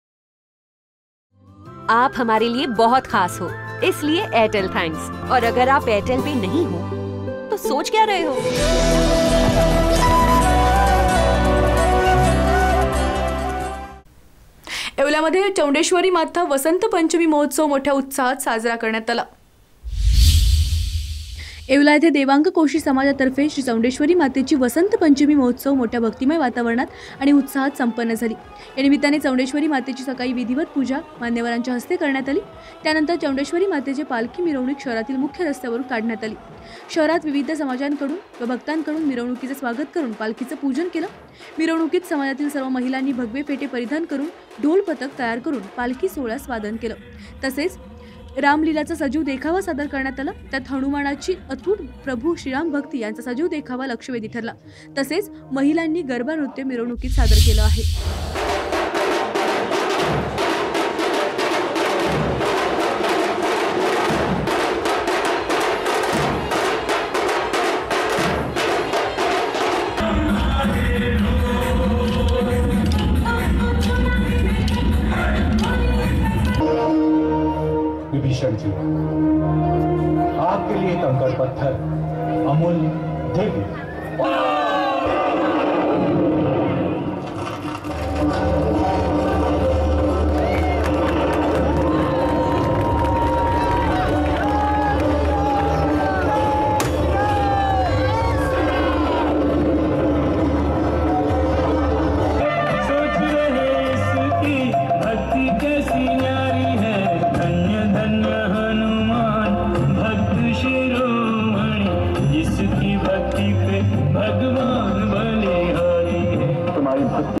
येवल्यात श्री चौंण्डेश्वरी माता वसंत पंचमी महोत्सव उत्साहात संपन्न
येवल्यात श्री चौंण्डेश्वरी माता वसंत पंचमी महोत्सव उत्साहात संपन्न, रामलीला सजीव देखावा आणि महिला ढोल पथकाने वेधले लक्ष.@